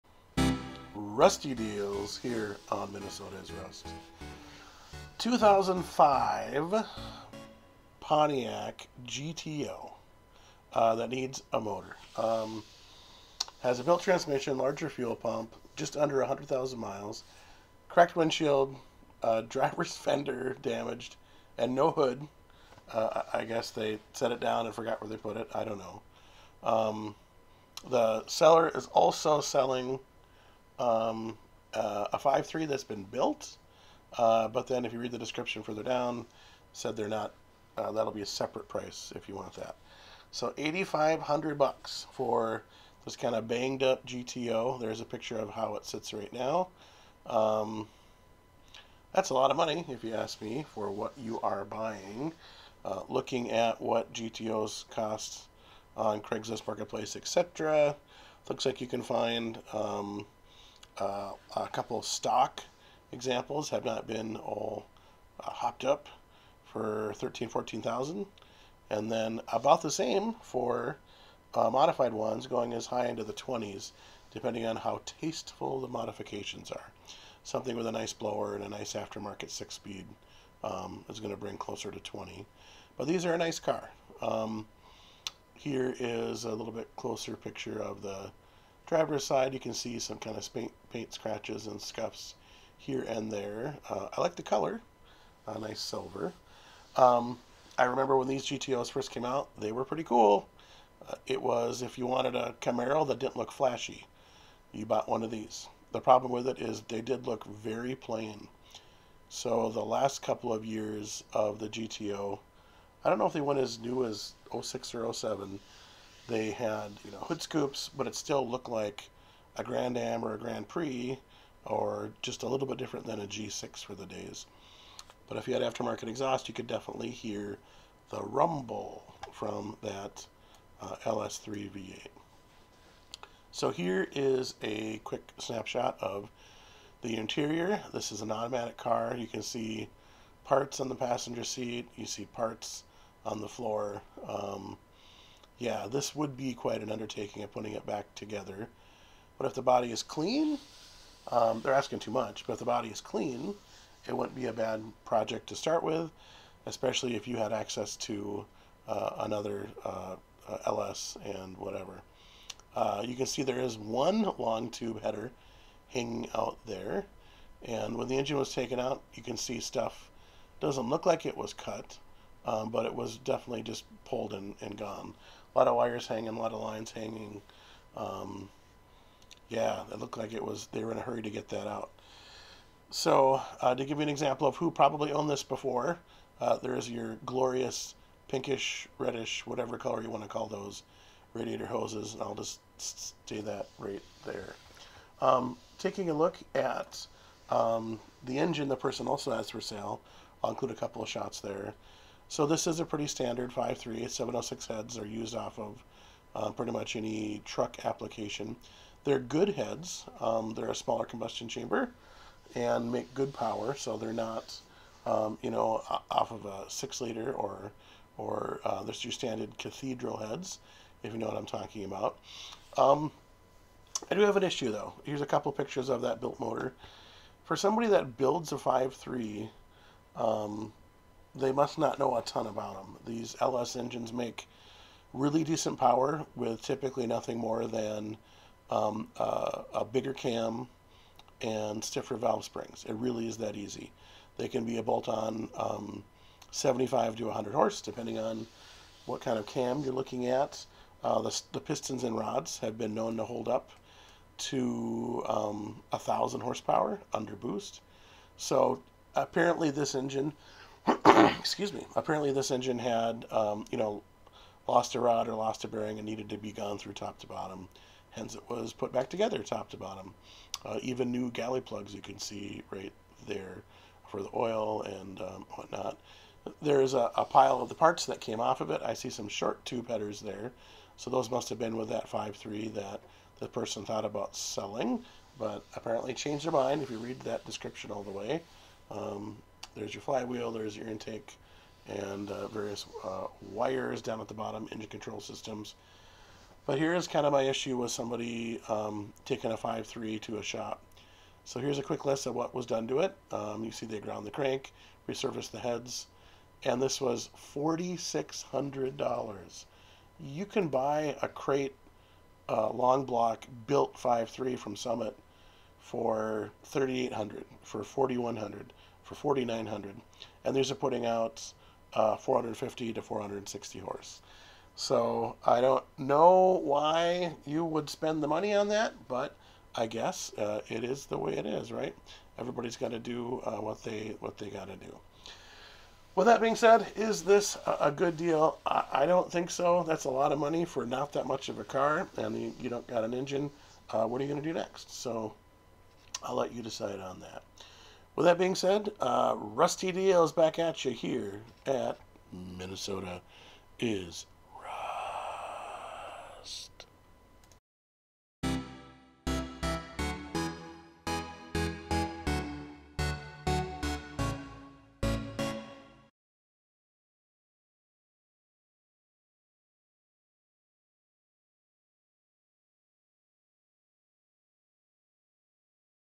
MISSING A MOTOR 2005 Pontiac GTO Marketplace Project: RUSTY DEALS 13
We spend too much time looking at cars and trucks for sale on all the online marketplaces and auction sites. Sometimes we come across something that catches our eye. It all comes down to one question: do you want this thing rusting away in your backyard or someone else's?